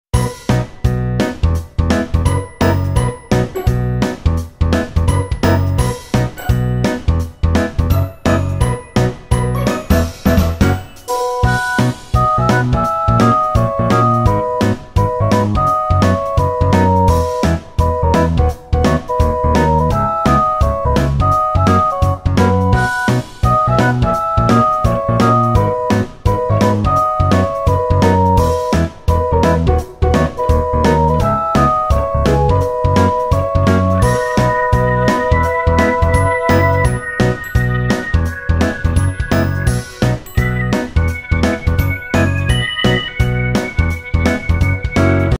Easy Pumpkin Playdough Recipe
Kids will love this pumpkin pie scented playdough, it's perfect for fall! This homemade playdough is a no cook recipe and super easy!